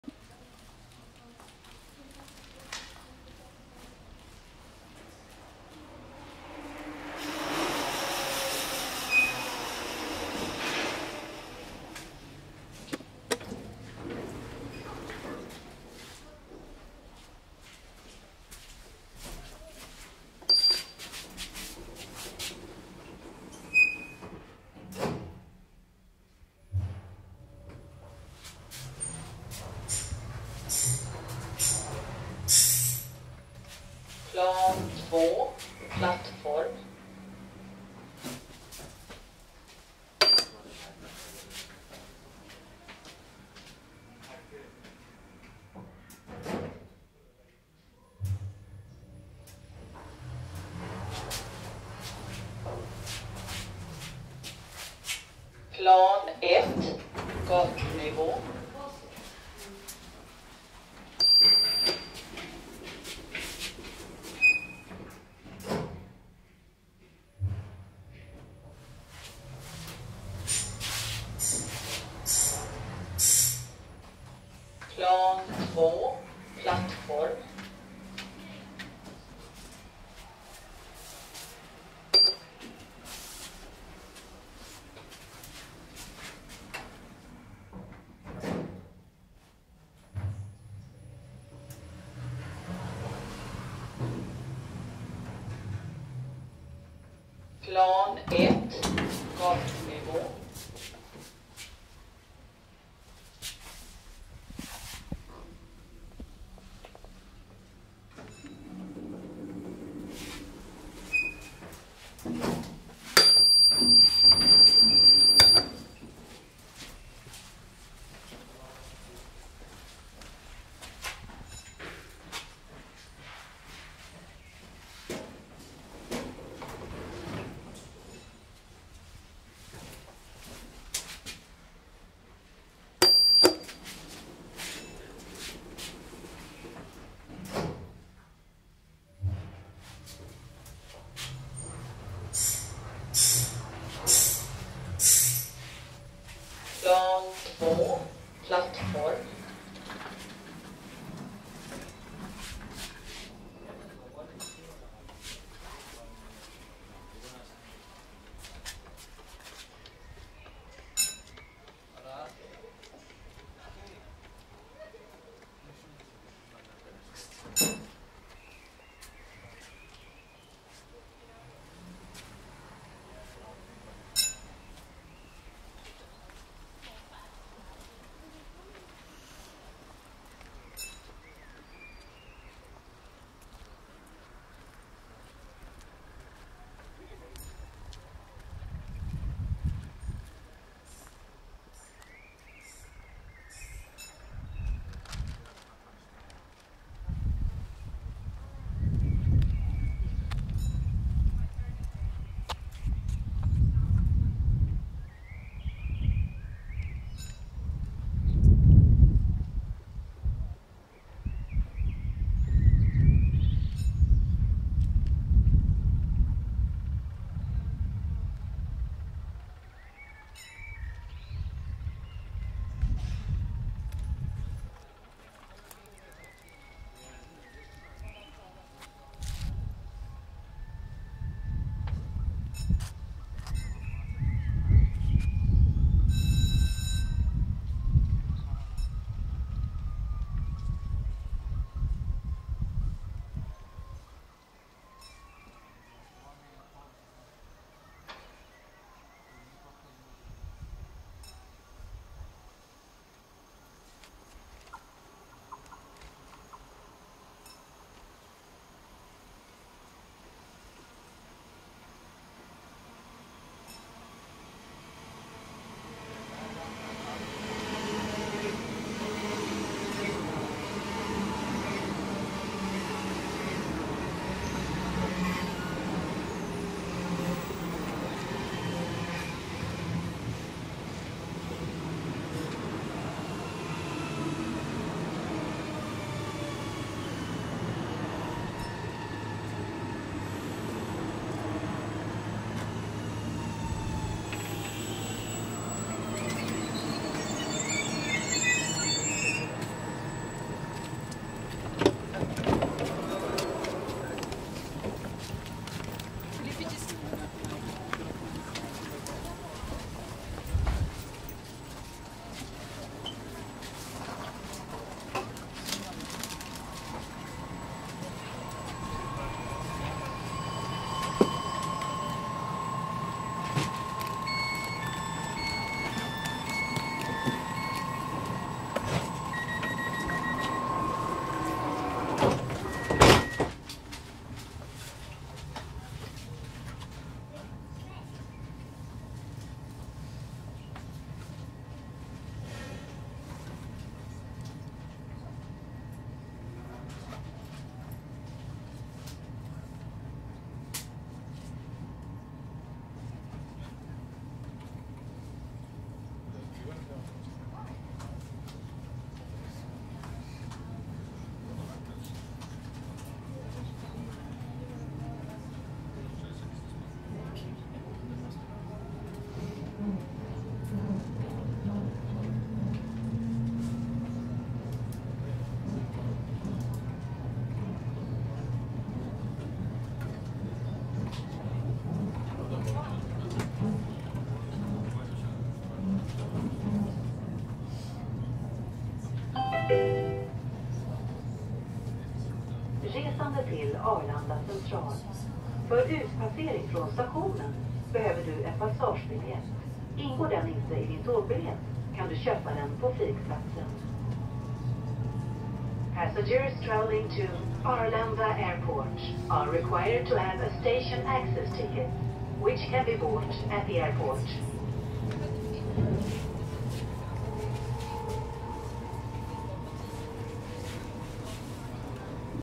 2010 Uppsala Hiss AB MRL Traction Elevator @ Knivsta Railway Station, Knivsta, Sweden.
Recorded: 14/6-24.
Camera: iPhone 13 Pro Max.
Microphone: Shure MV88.

To platform.
Brand: Uppsala Hiss AB.
Built: 2010.
Type: MRL Traction.
Capacity: 1000 KG or 13 Persons.
Floors: 2 ( *1* 2)
Location: Knivsta Railway Station, 741 40 Knivsta, Sweden.